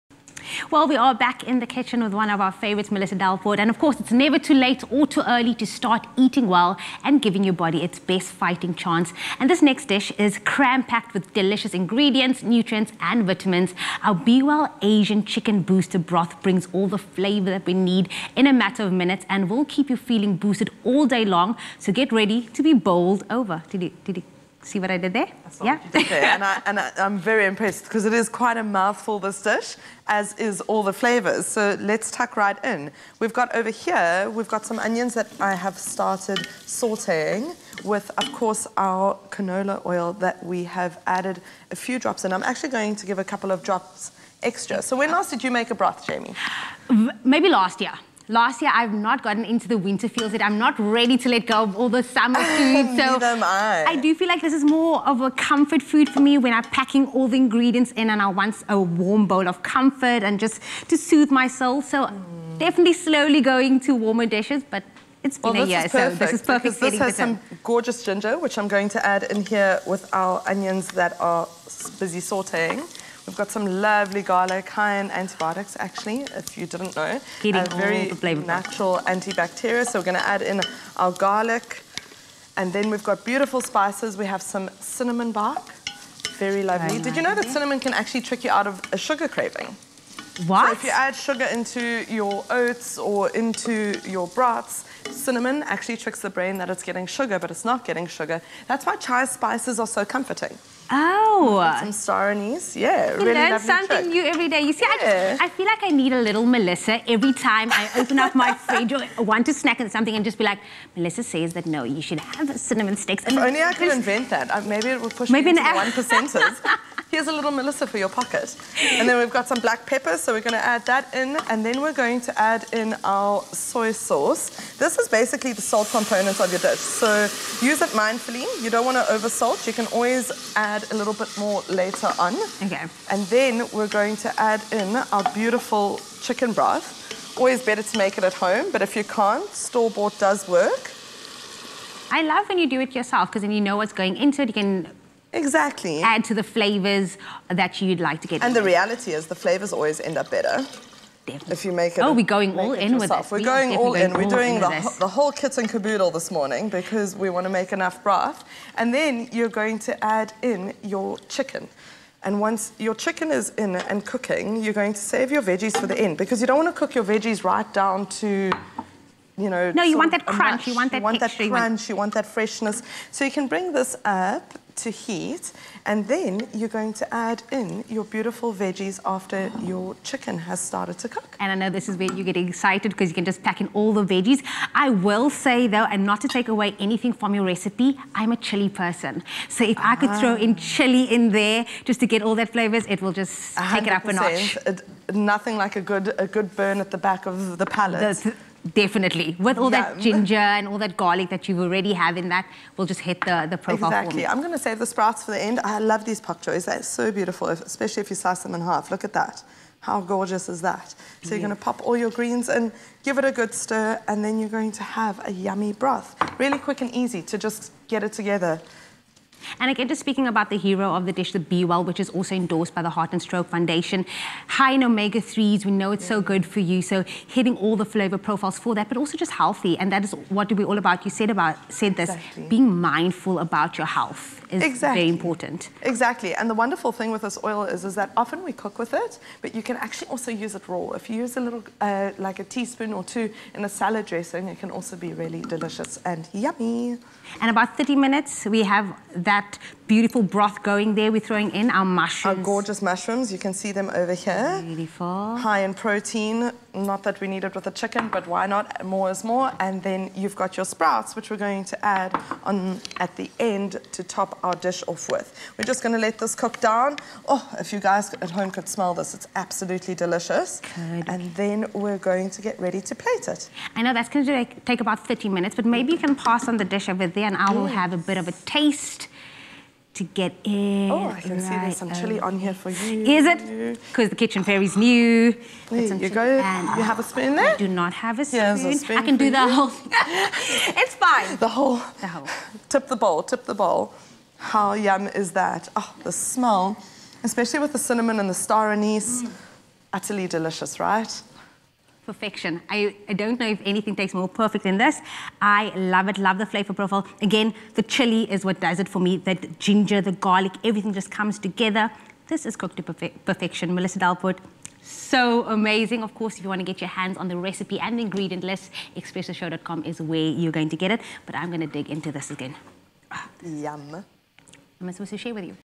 Recipe: Asian Chicken and Veg Booster Broth (BWELL)
In light of World Health Day, we’ve got a dish that’s packed with flavour, nutrients, and the goodness of B-well Canola Oil! Please your tastebuds, and give your immunity a boost with this delicious Asian Chicken and Veg Broth!